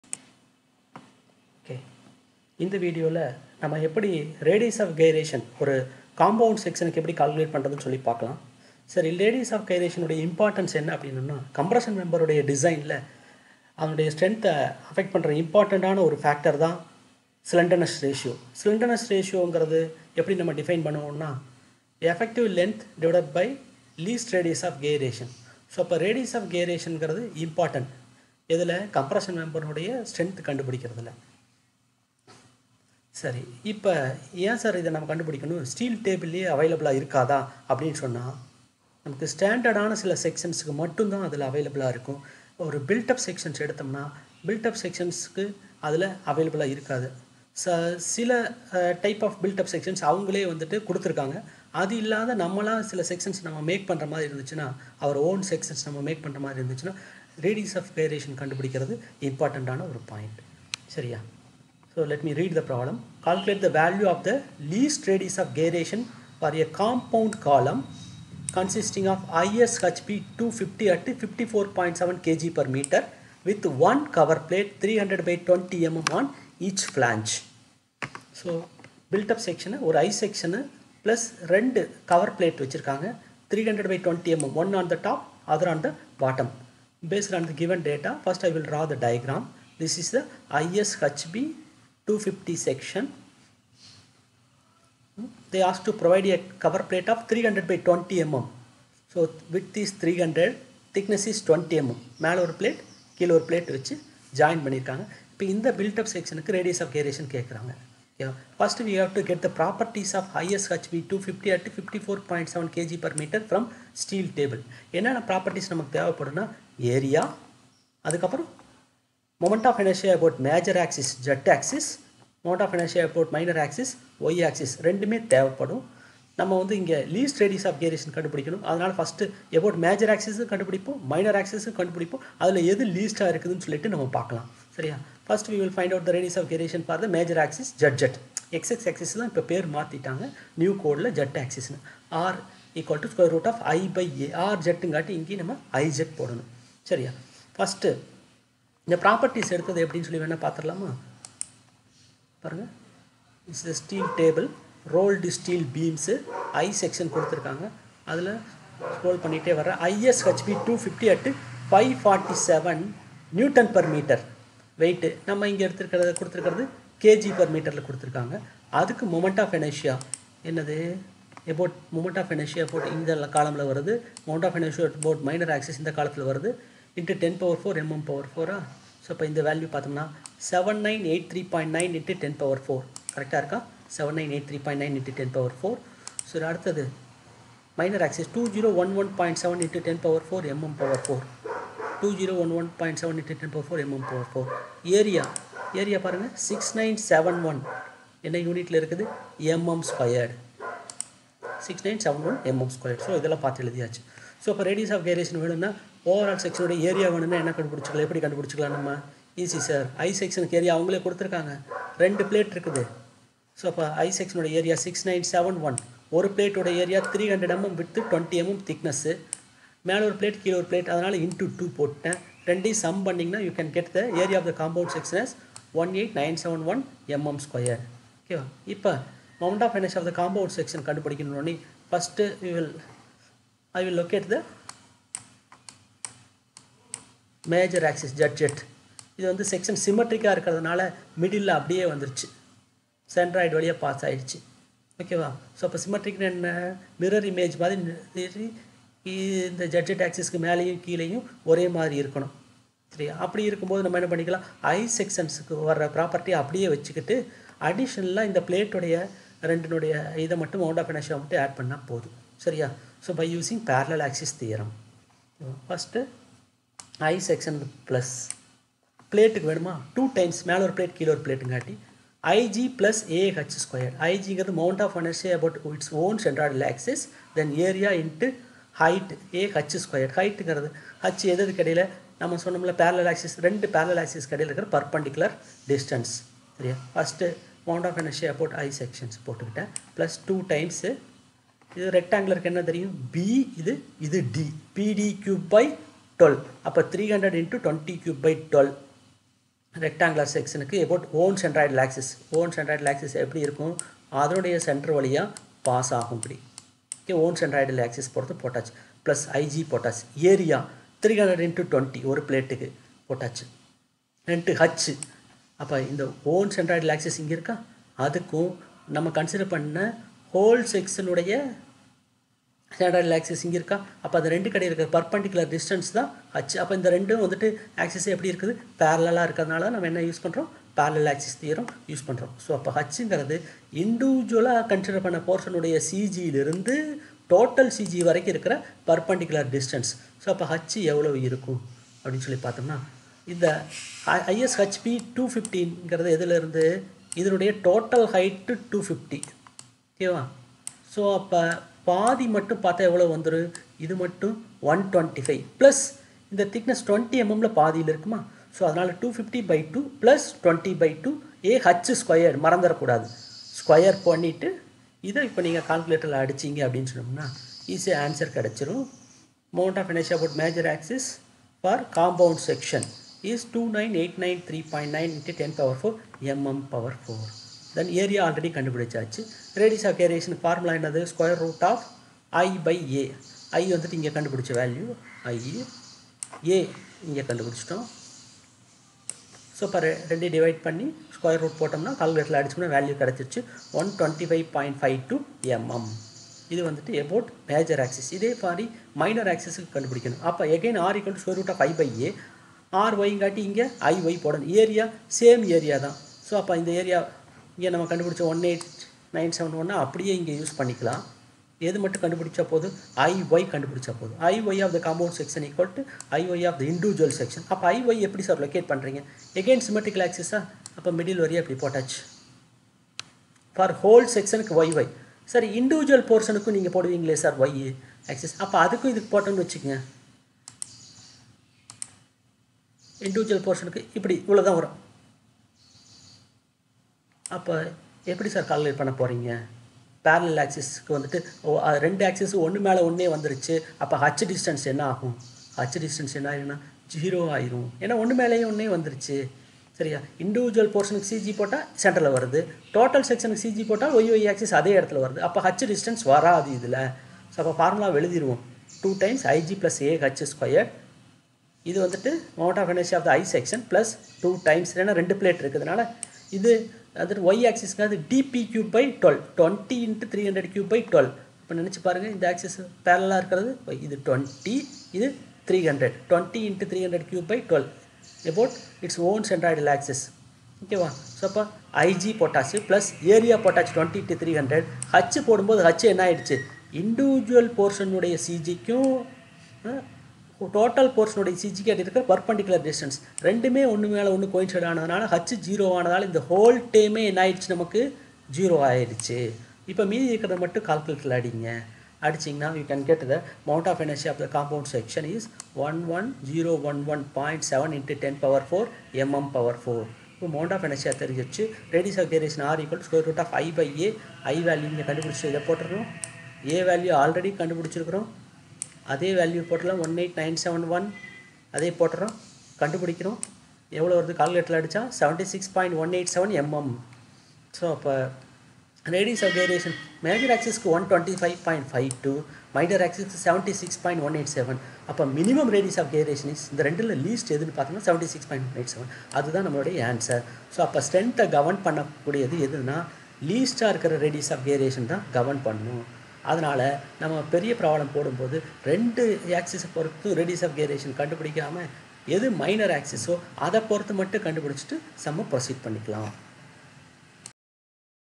Radius of gyration | Compound column | Tamil | Steel compression members
This video explains how to find radius of gyration of compund/built-up sections.